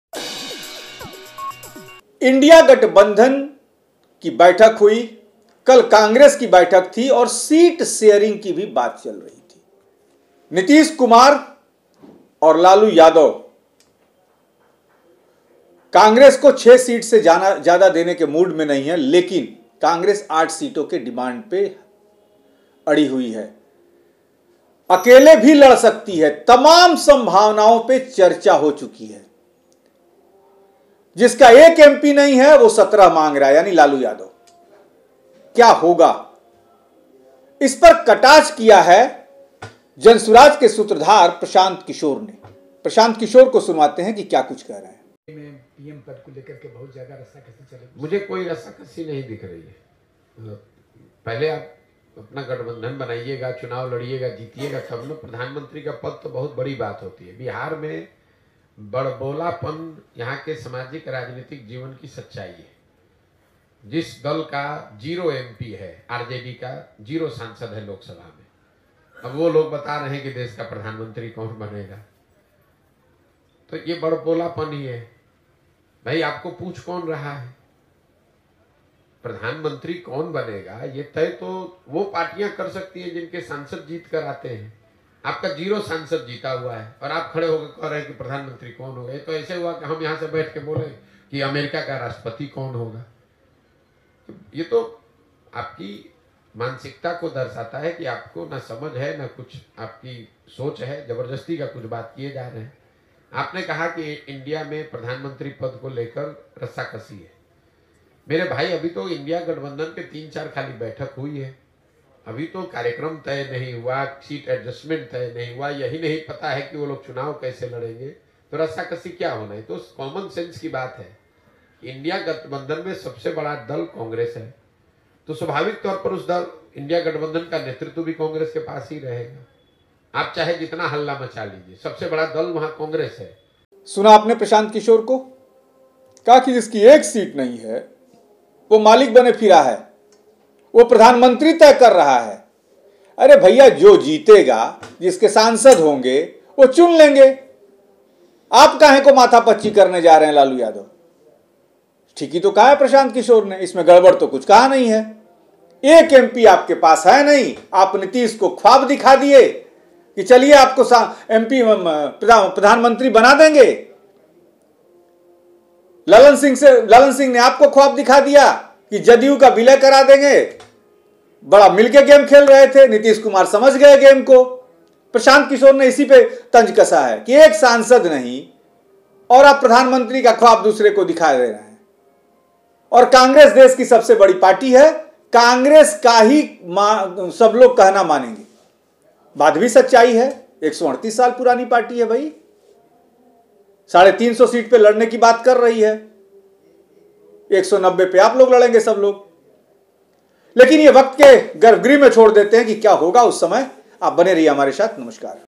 राजद और जदयू में चल रही है रस्सा-कस्सी, प्रशांत किशोर ने बता दी अंदर की बात
राजद और जदयू में चल रही है रस्सा-कस्सी
प्रशांत किशोर ने बता दी अंदर की बात

bihar news
bihar news today
patna news